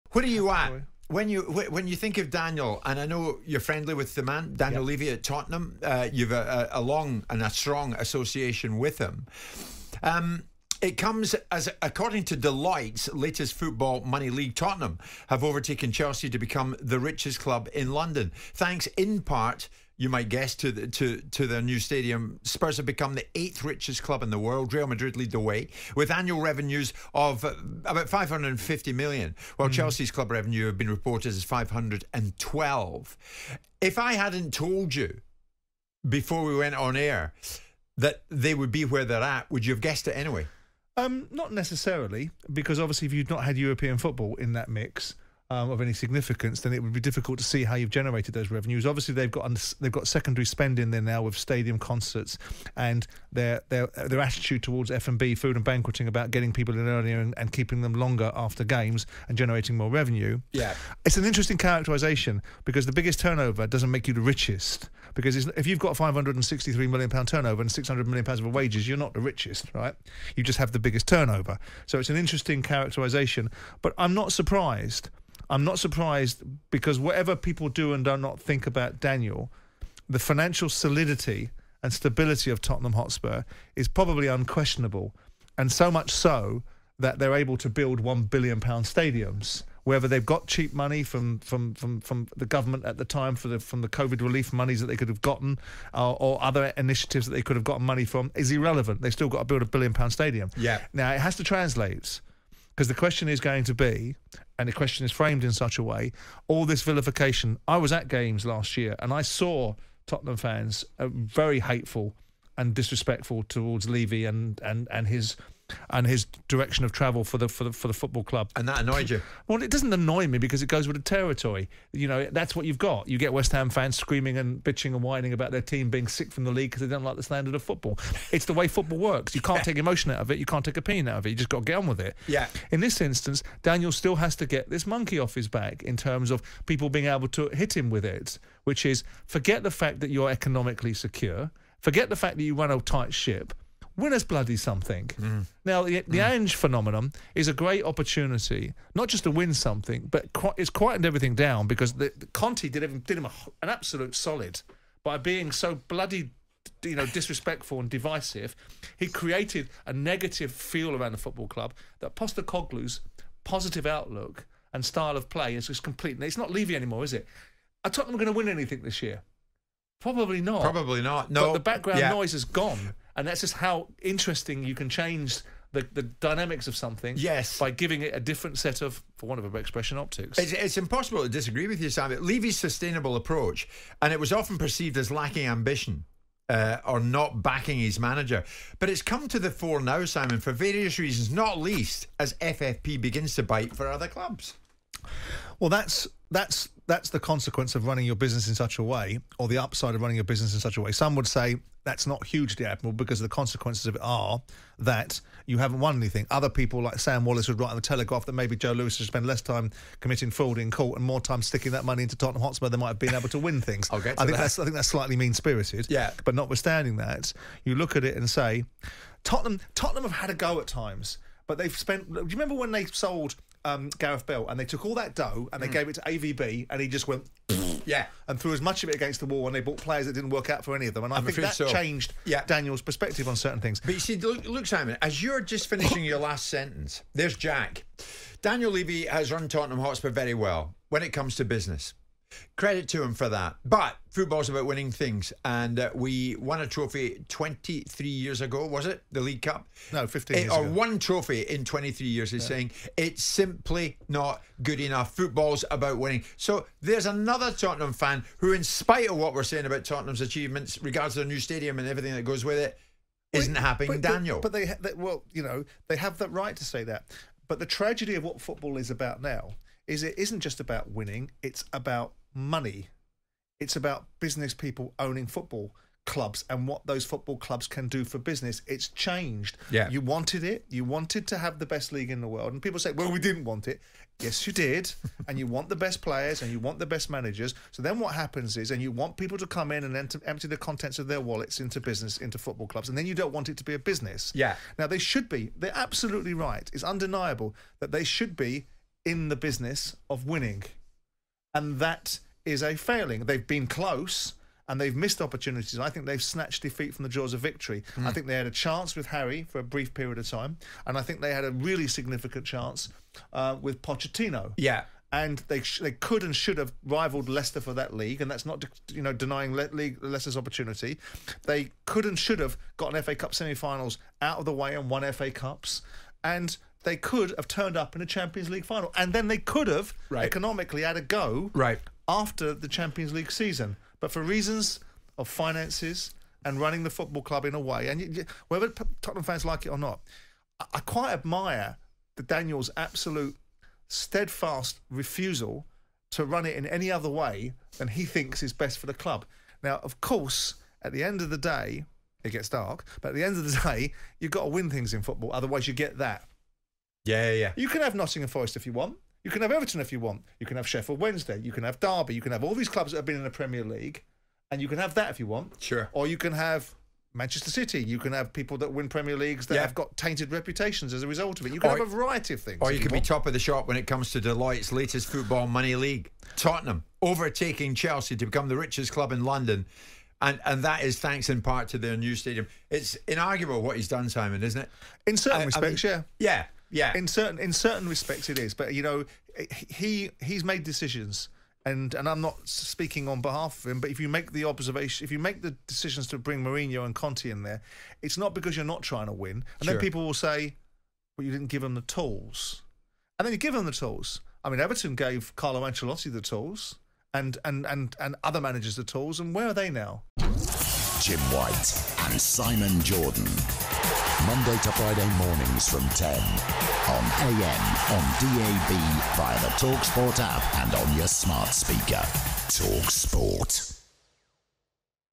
Simon Jordan REACTS To Spurs Being London's RICHEST Club & URGES Daniel Levy To Now WIN Trophies 💰🏆
TalkSPORT's Simon Jordan shares his thoughts on Tottenham Hotspur becoming the richest club in London.